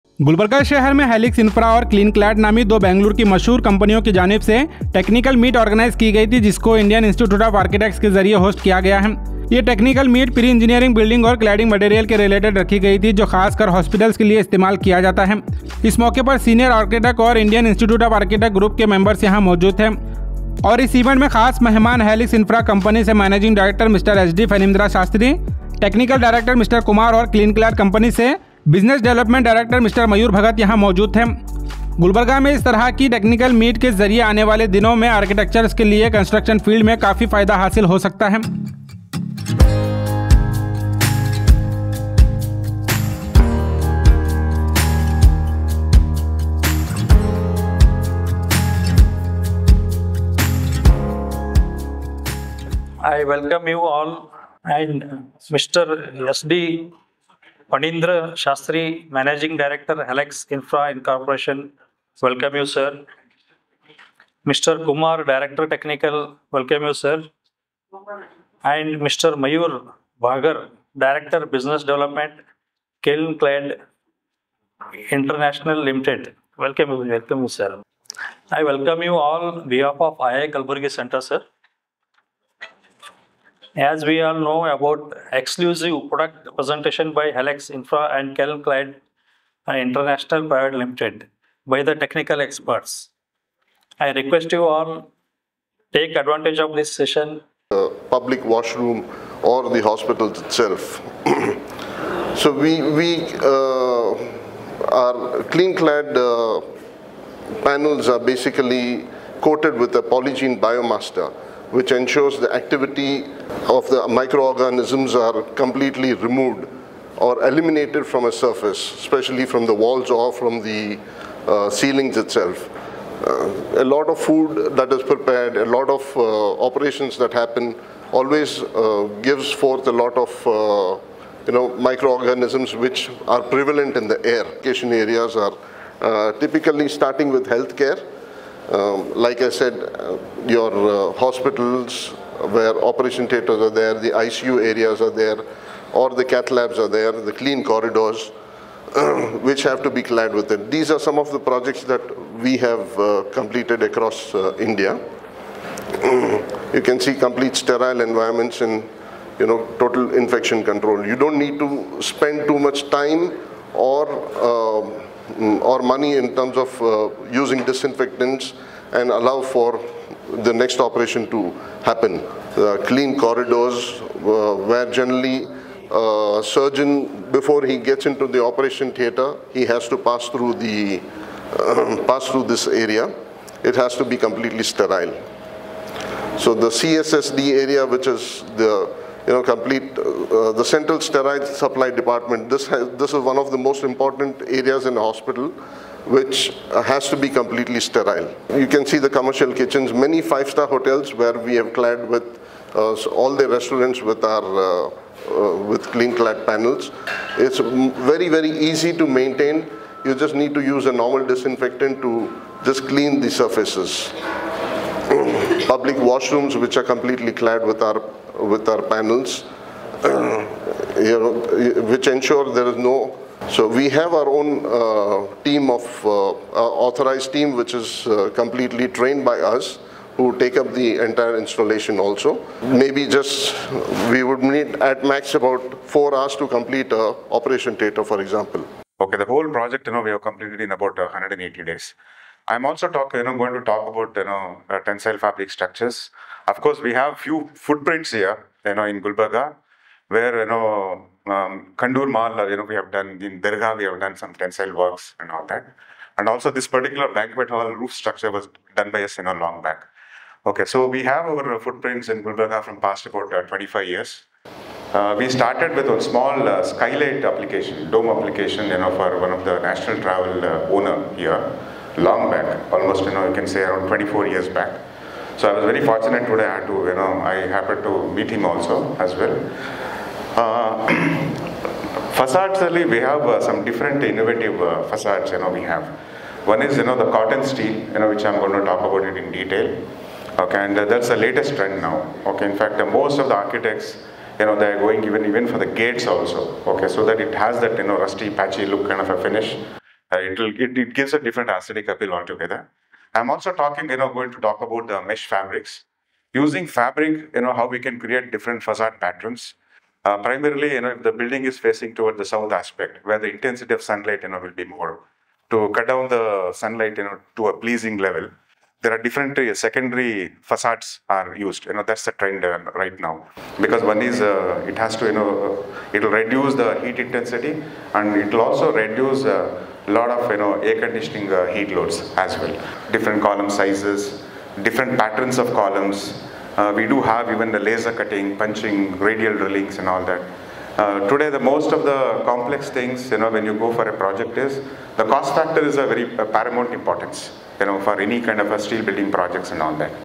Gulbarga: Helix Infra and KleenClad Organize Technical Meet Hosted by Indian Institute of Architects
Helix Infra and KleenClad Organize Technical Meet in Gulbarga, Hosted by Indian Institute of Architects - Focus on PEB and Cladding Solutions for Hospital Construction